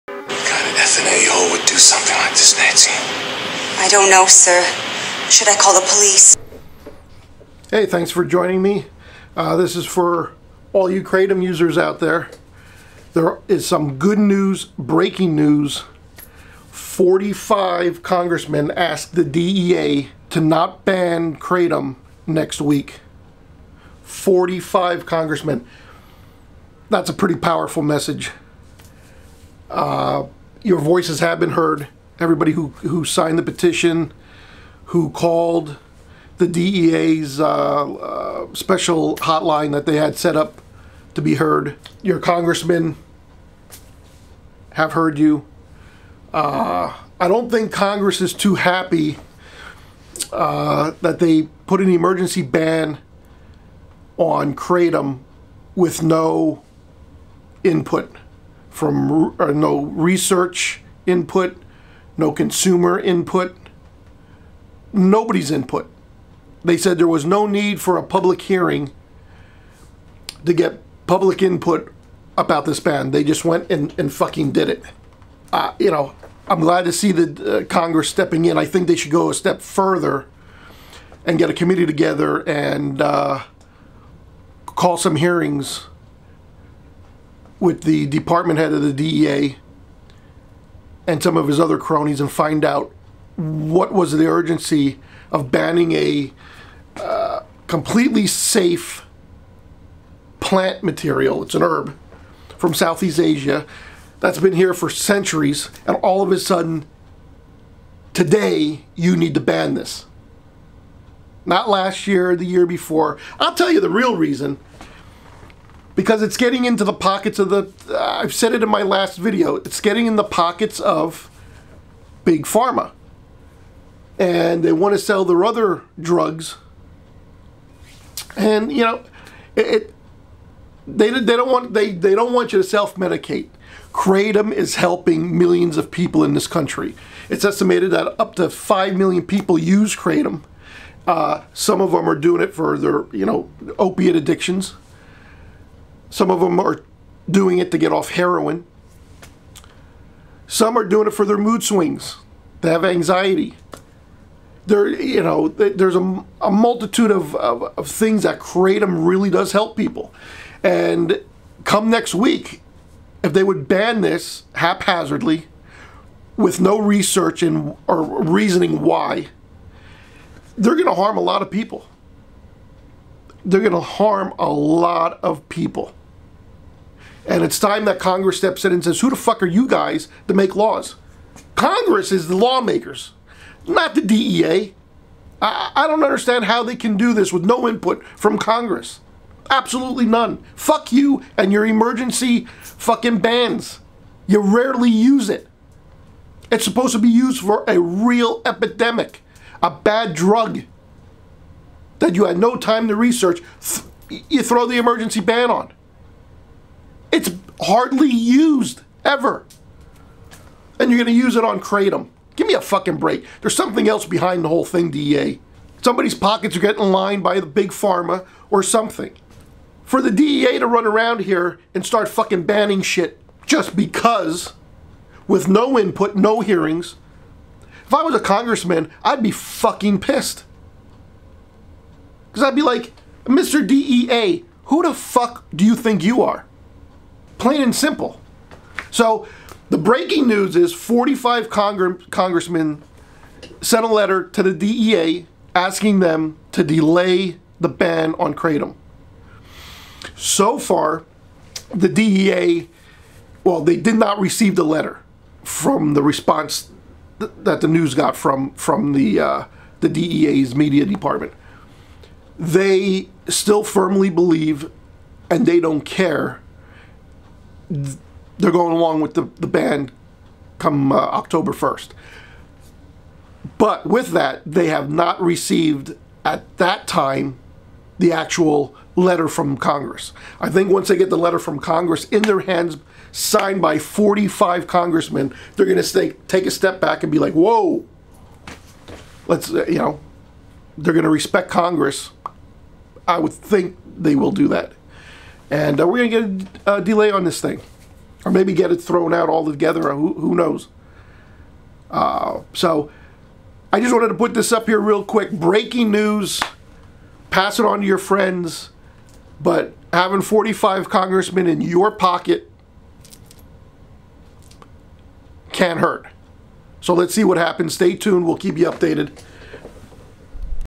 BREAKING NEWS: Congress to DEA - Delay Ban on Kratom!
45 Congressmen send letter to DEA head asking him to delay ban on Kratom!  That's great news!